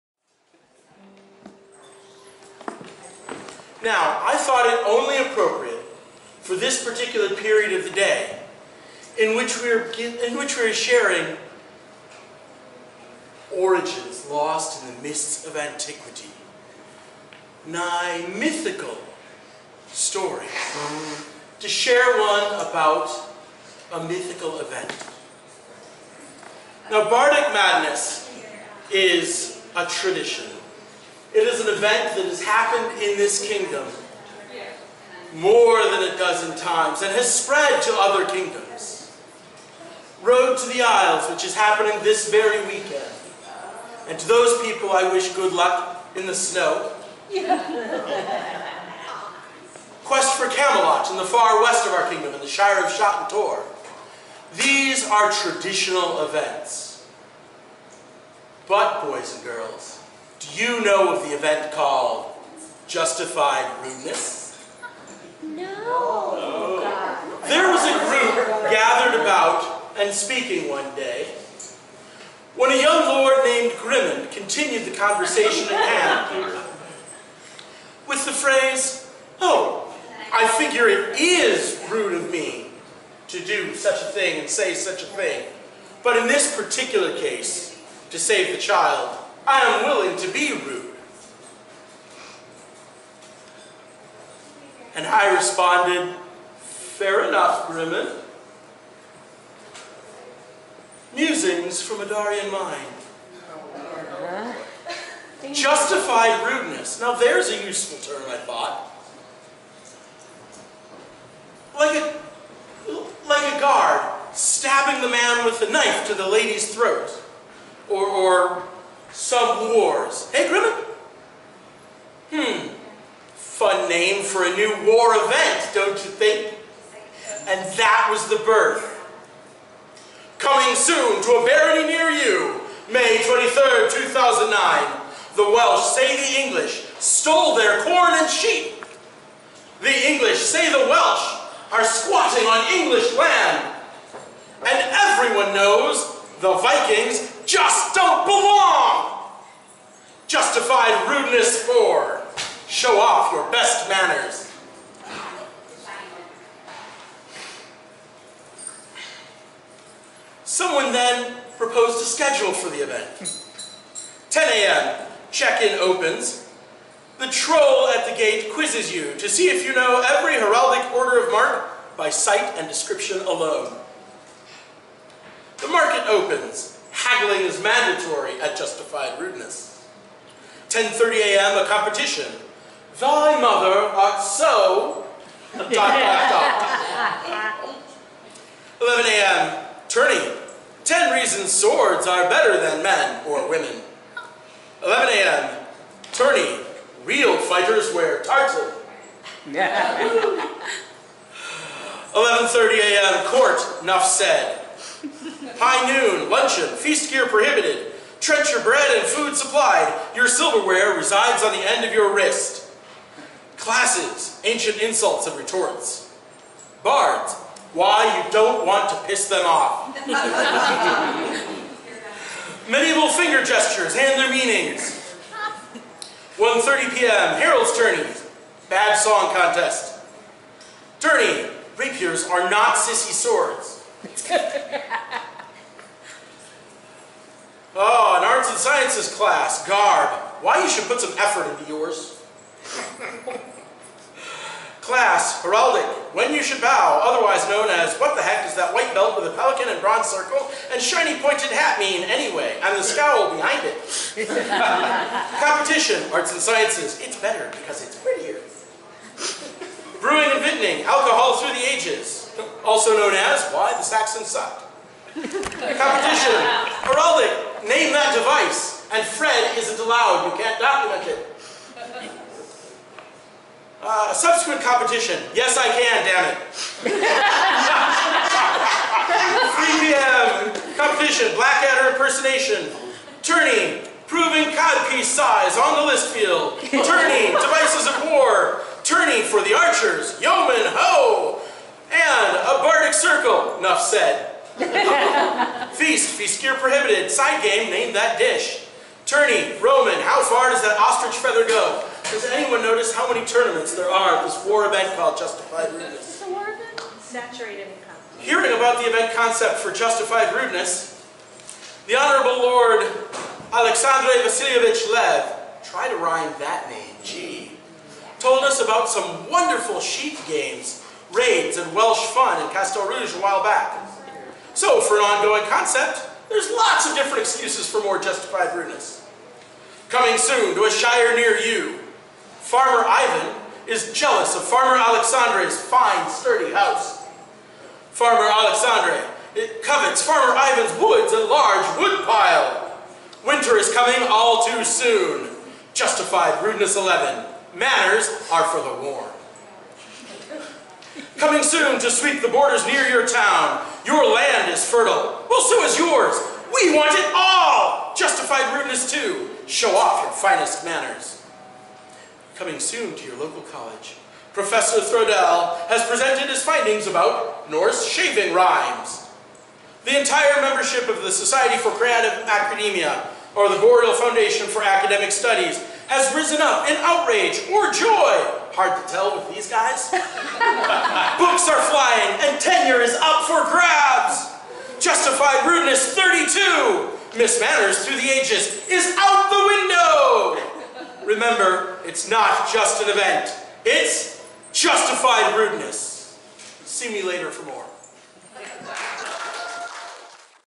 Mythunderstandings -- Dahrien
Dahrien answers the first of many challenges. Mythunderstandings: "Myths and legends help people understand how things may have come to be, but the creation of mythologies need not be restricted to the ancients. Consider a period or SCA tradition or item of mysterious origin and present a new myth to explain how it came to be."

Recorded at Bardic Madness in the Barony of Caer Anterth Mawr on April 26, 2014.